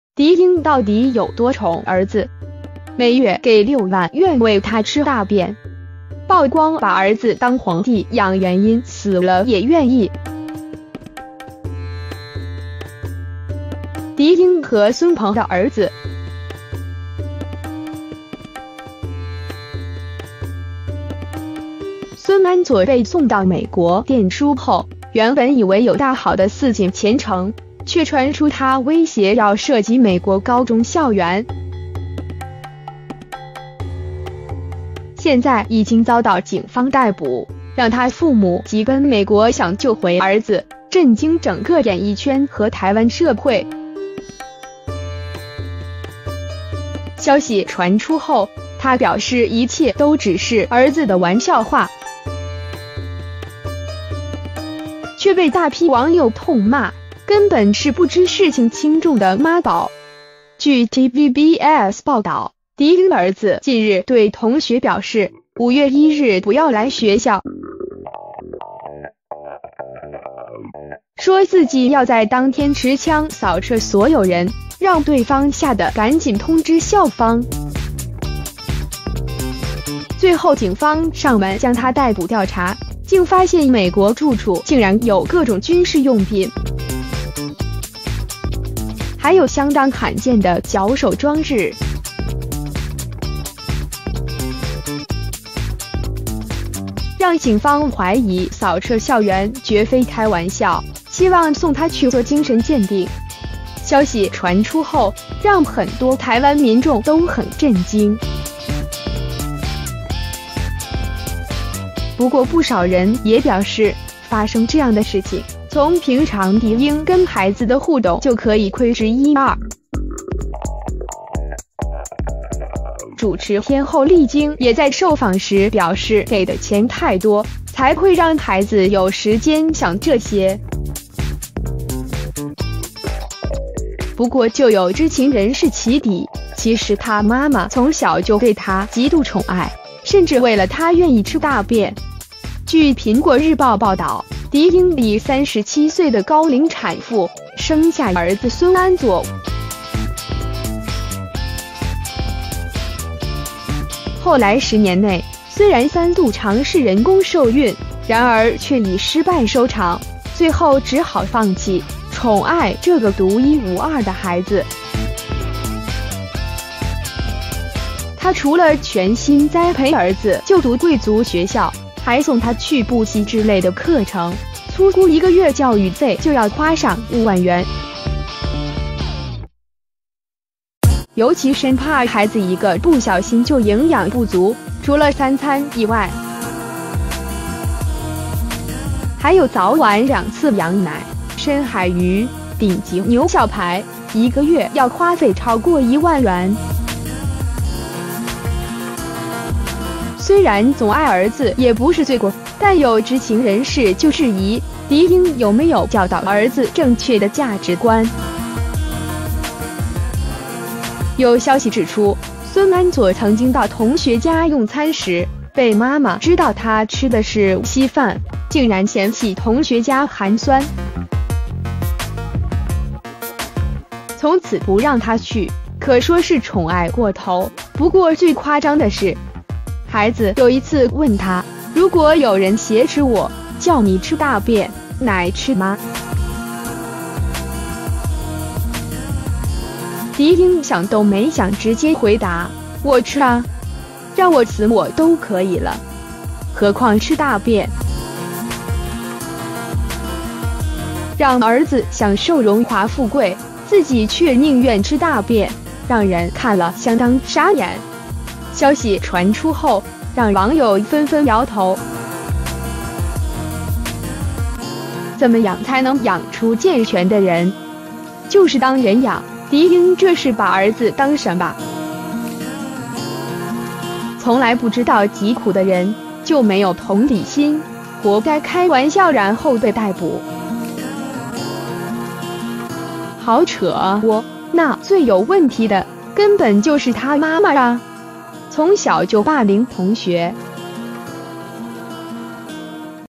狄鶯到底有多寵兒子！「每月給6萬」願為他吃大便！曝光「把兒子當皇帝養原因」死了也願意！
狄鶯和孫鵬的兒子，

孫安佐被送到美國唸書後，原本以為有大好的似錦前程，卻傳出他威脅要涉及美國高中校園，

現在已經遭到警方逮捕，讓他父母急奔美國想救回兒子，震驚整個演藝圈和台灣社會，

消息傳出後，她表示一切都只是「兒子的玩笑話」，

卻被大批網友痛罵：「根本是不知事情輕重的媽寶！」